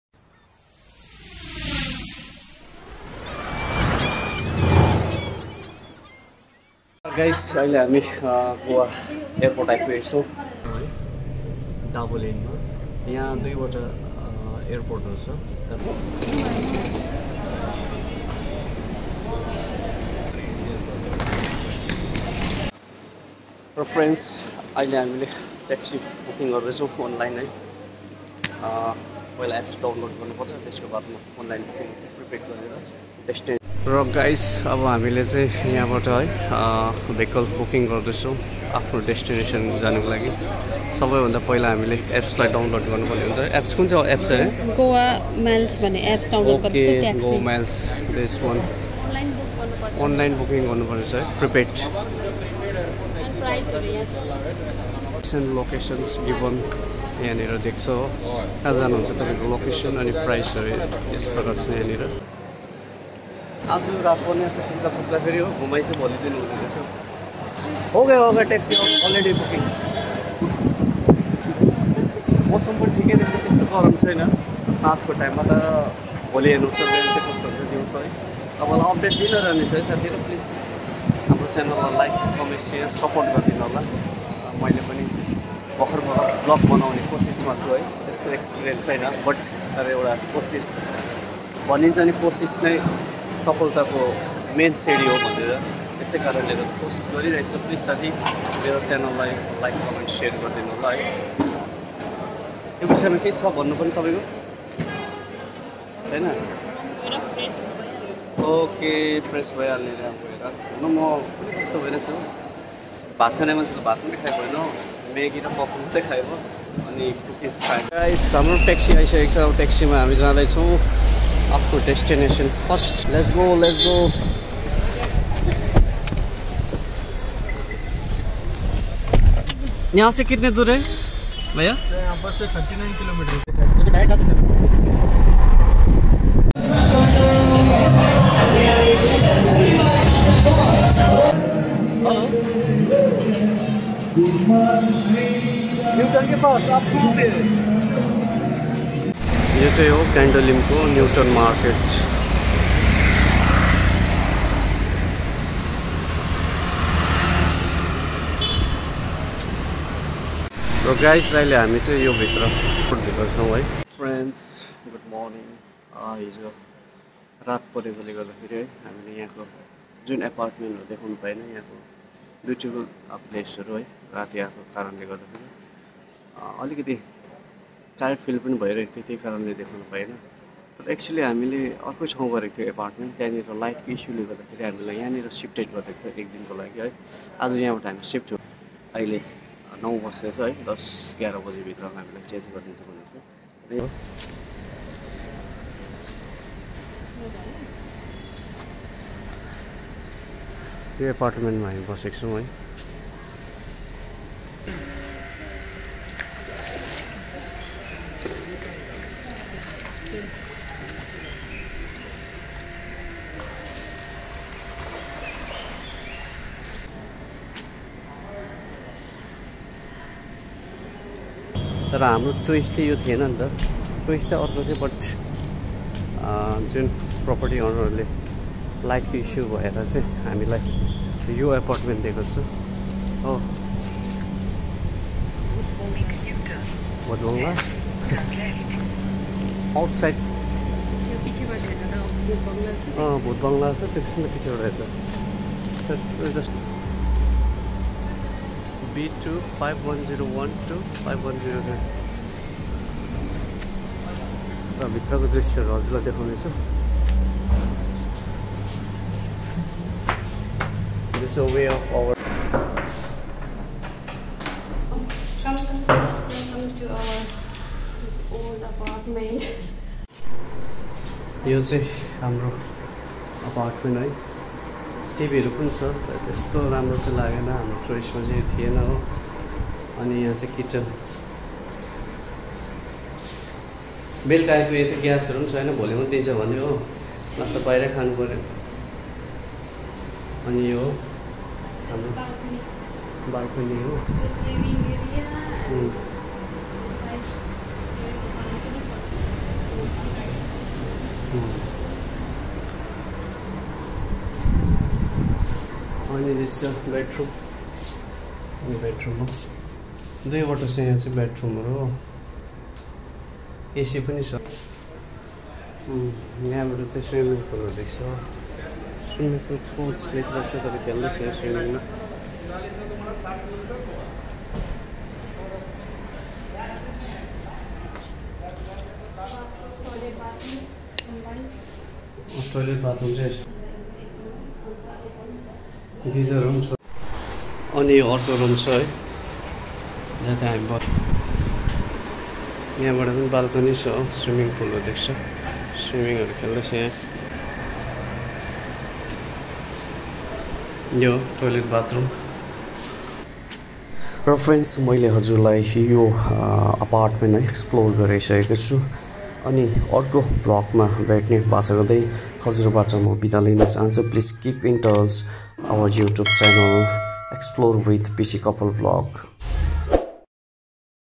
Exploring Goa, Candolim Vlog/# Let me show you, our accidental Apartment in Candolim North Goa.
Firstly, we stayed near by Newton market of Candolim North Goa.

Support our Youtube Channel.

Candolim North Goa
Newton super Market
Candolim Beach

#"Short information about Candolim North Goa, India".

Candolim is a village in North Goa, India.It is situated just South of the famous Calangute beach,and is a popular tourist destination.
Besides a place of tourist, Candolim has historic significance as well.The  founder of Science of hypnotism, father Abbe Faria was born here.Candolim is also the place where the Goans launched first revolt against Portrugese.Predominantly Catholic in Nature but all relegion are catered for here.
Note: futher more information can get from Google.Thank you.